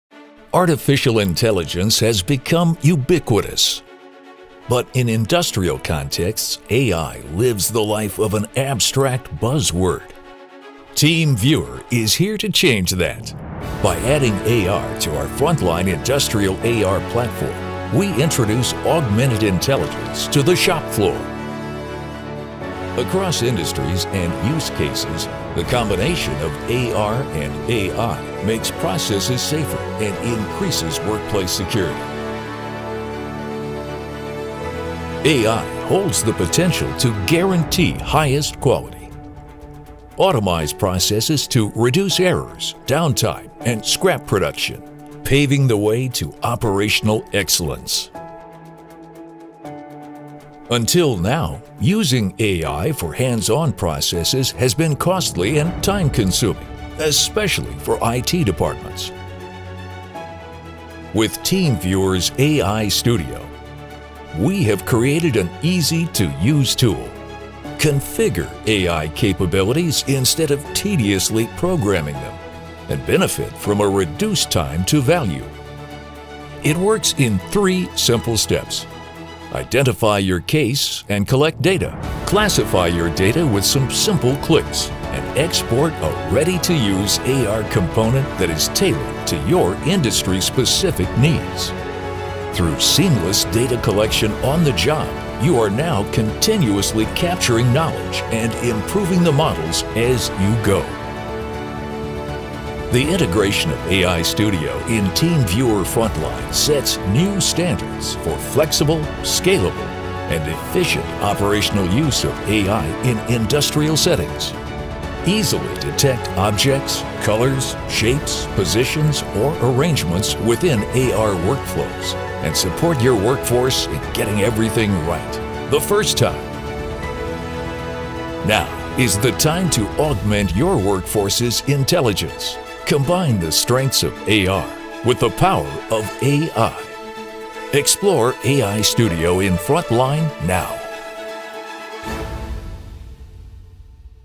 Combining the strengths of AR with the power of AI: Introducing Ai-Studio add-on in Frontline
We are excited to introduce the next logical step when it comes to empowering frontline workers: Adding AI capabilities to the shopfloor with our AiStudio add-on for Frontline.
By combining the strengths of AR with the power of AI we are creating an actionable AI expereince for our customers - across use cases and industries. This is a game changer to open up massive untapped potentials when it comes to streamlining manual processes and improving industrial workplace safety.
And on top of that, we created AiStudio to be a self-service tool! With a few simple clicks you immediately become an AI expert and create image and object recognition capabilities that are ready-to-use within Frontline AR solutions.